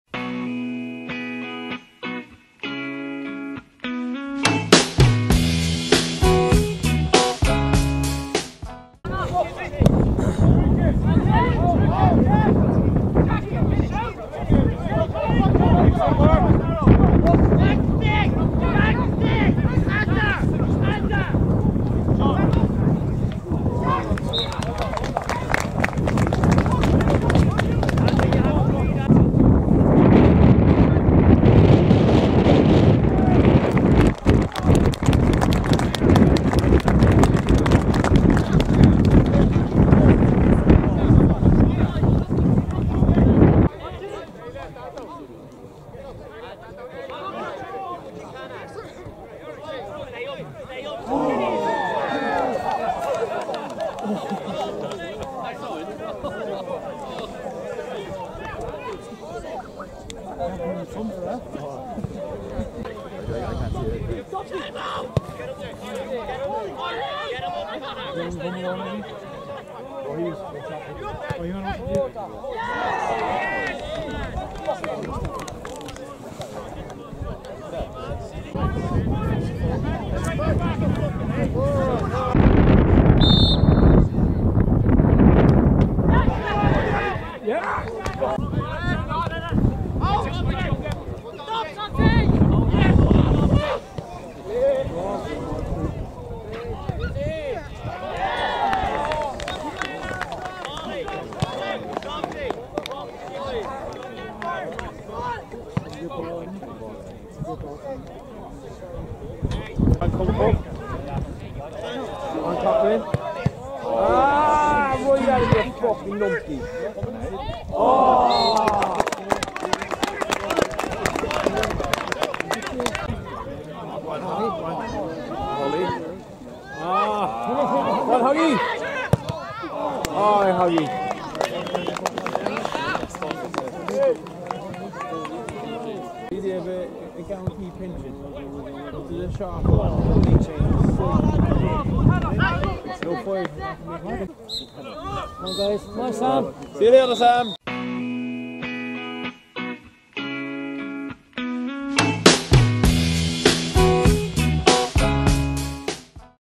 Cork City FC v Longford Town FC Highlights 02/02/19 | Pre-Season Friendly
Cork City FC 1-3 Longford Town FC
Saturday 2nd February 2019
Lotamore, Mayfield
KO: 2PM